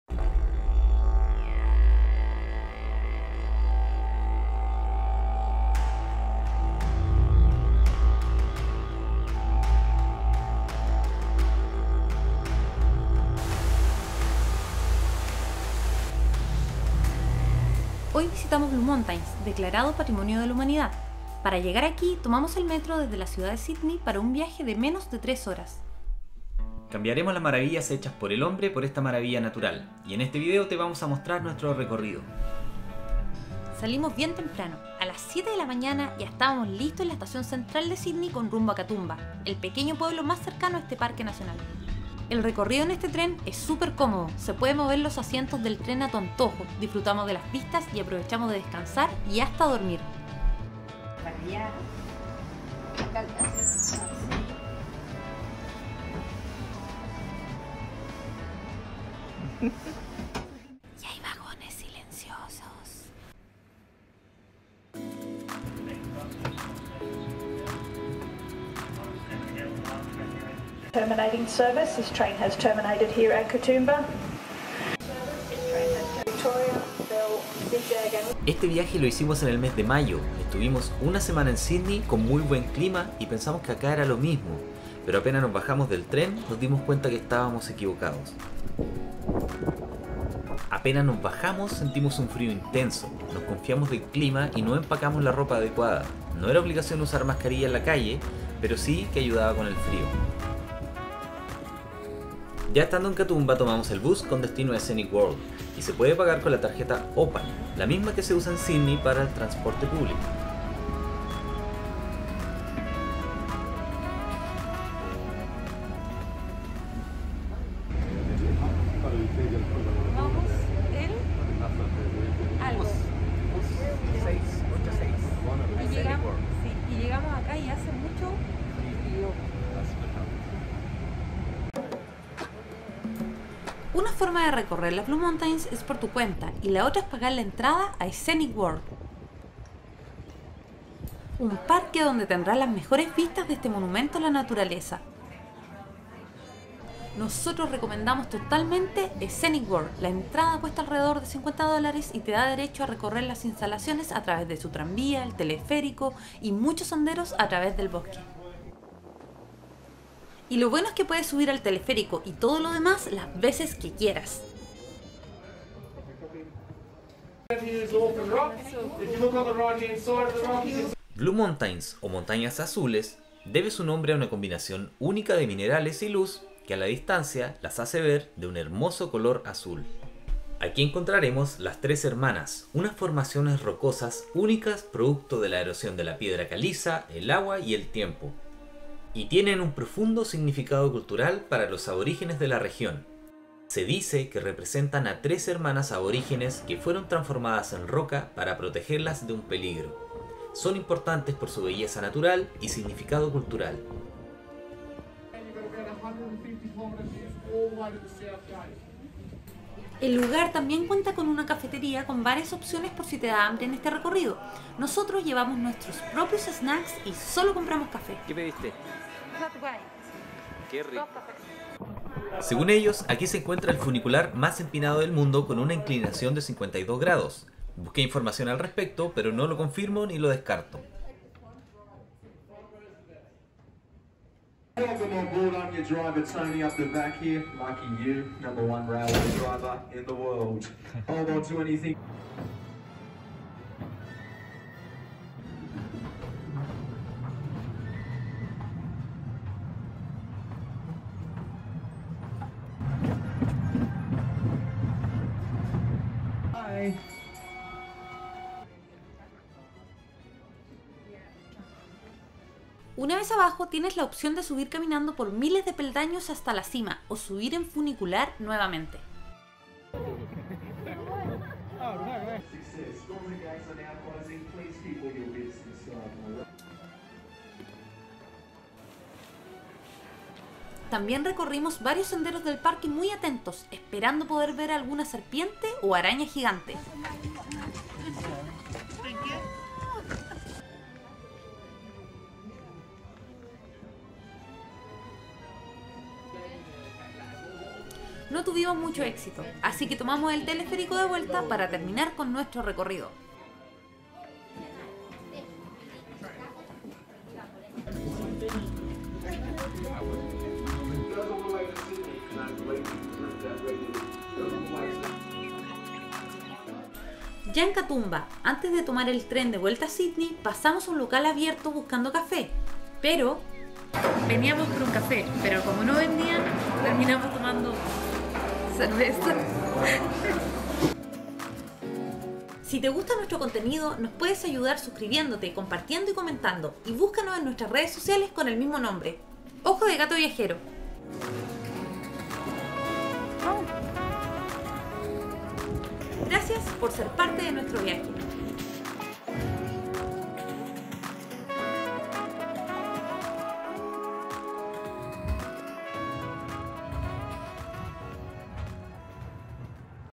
Visitando las Blue Mountains en Australia
Aquí te mostramos nuestro recorrido hacia las Blue Mountains, el patrimonio de la humanidad a poco menos de 3 horas desde Sydney.